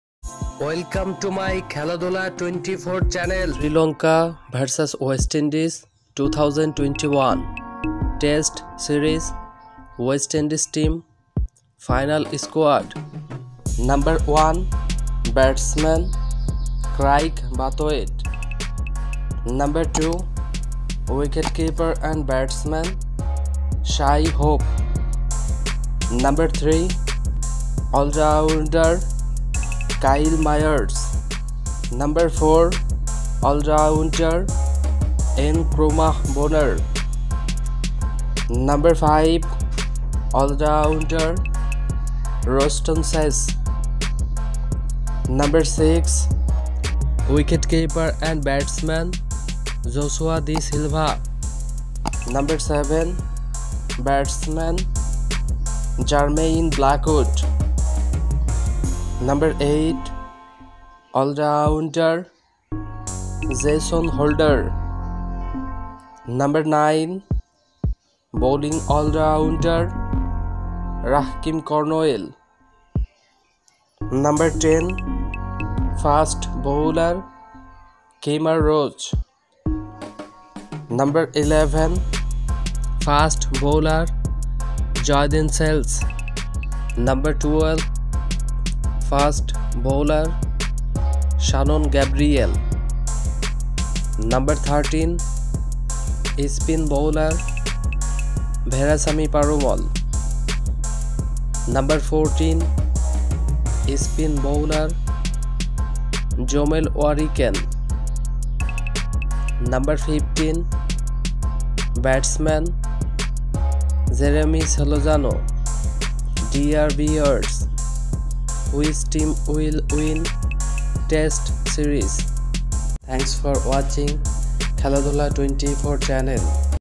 Sri Lanka Vs West Indies 2021 | West Indies Test Final Squad | SL vs WI Test Series 2021 Squad
West Indies squad:

Kraigg Brathwaite (c), Jermaine Blackwood (vc), Nkrumah Bonner, Roston Chase, Rahkeem Cornwall, Joshua Da Silva, Shannon Gabriel, Jason Holder, Shai Hope, Kyle Mayers, Veerasammy Permaul, Kemar Roach, Jayden Seales, Jeremy Solozano, Jomel Warrican.